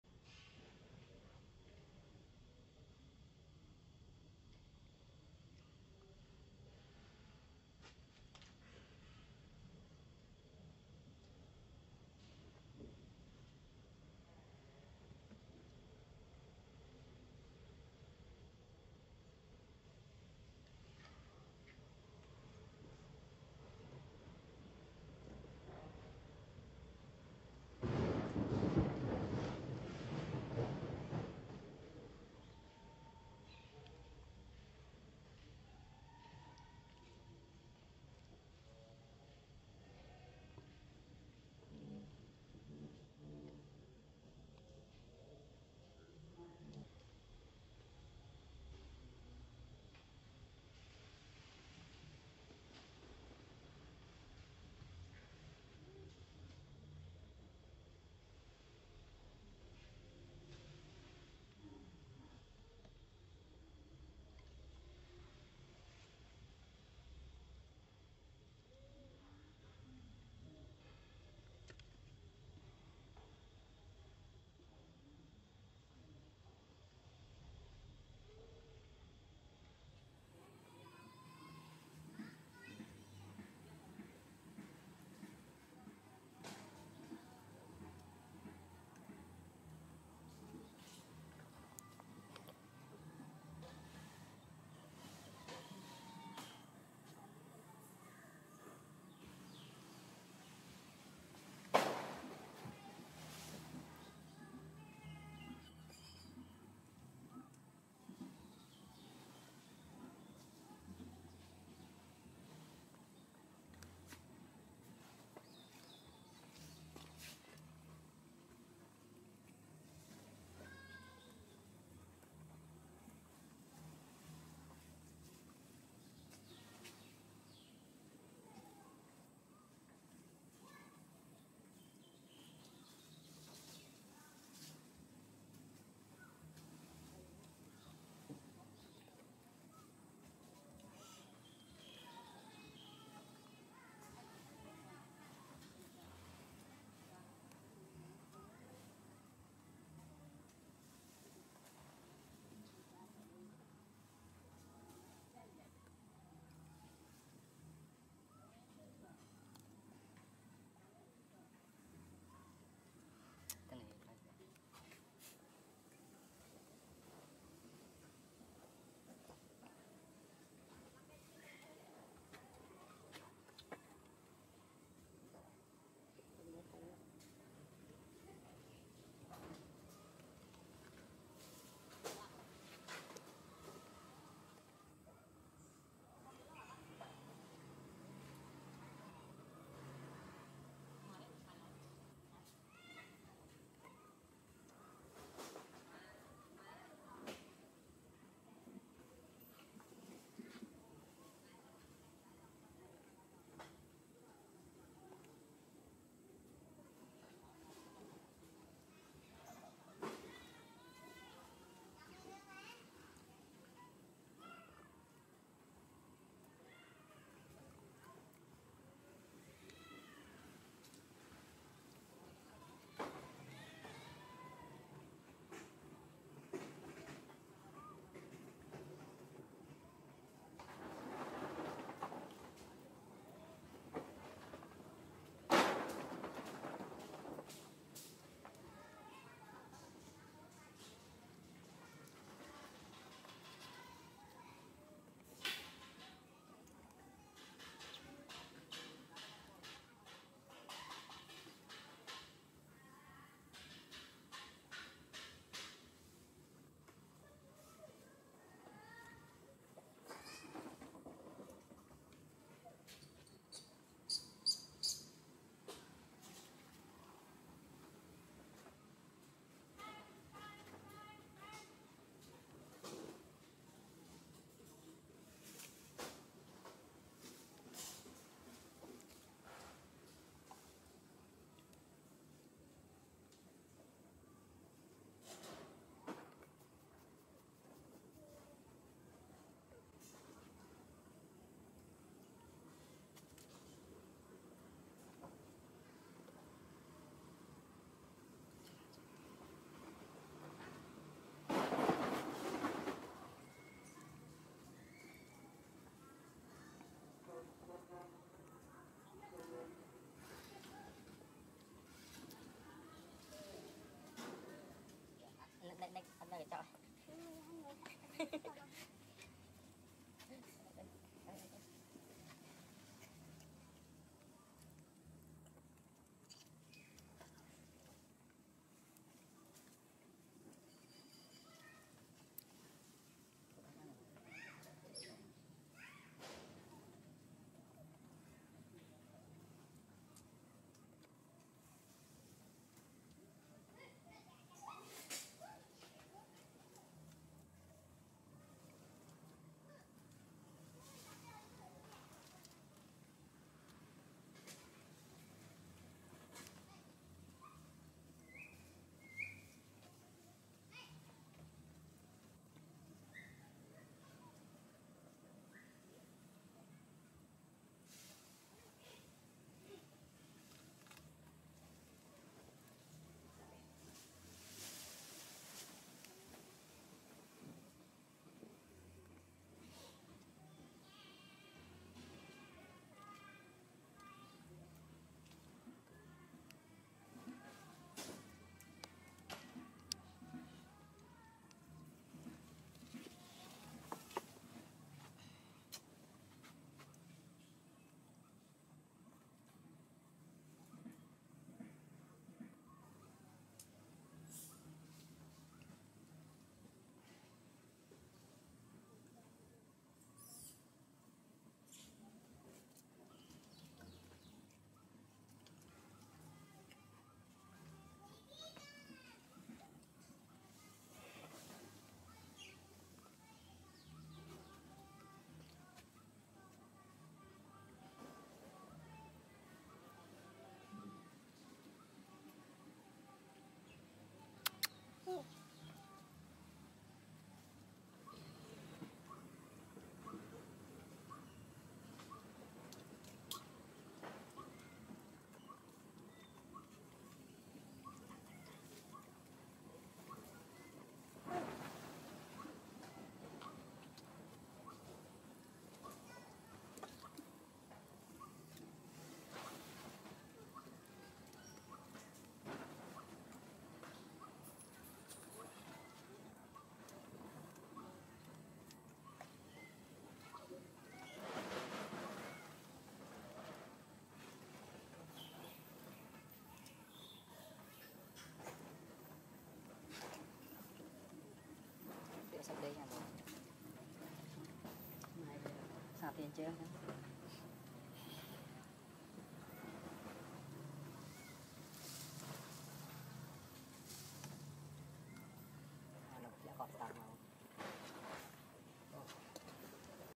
So sweetness! Mama Monkey Pinka brings Baby Monkey Pedro to learn to walk so cute and lovely so much
Title: So sweetness! Mama Monkey Pinka brings Baby Monkey Pedro to learn to walk so cute and lovely so much